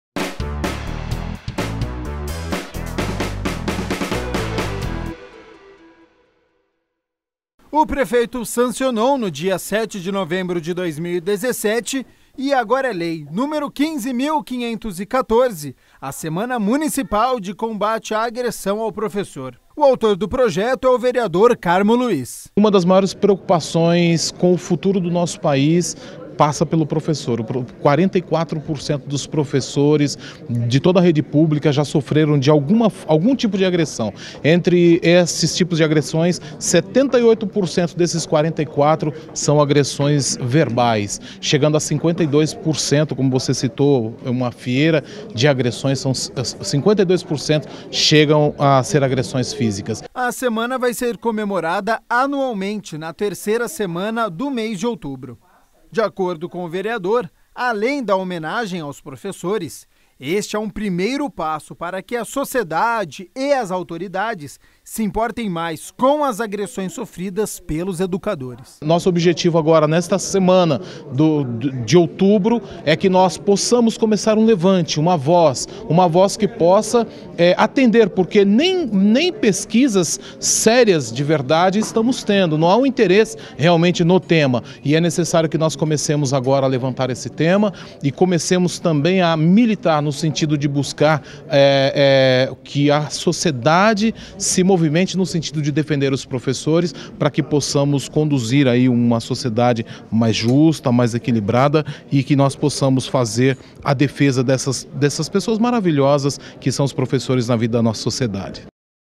Agora é Lei : Semana de Combate Agressão ao Professor 13/11/2017
Entrou no Calendário Oficial do Município,a Semana Municipal de Combate à Agressão ao Professor.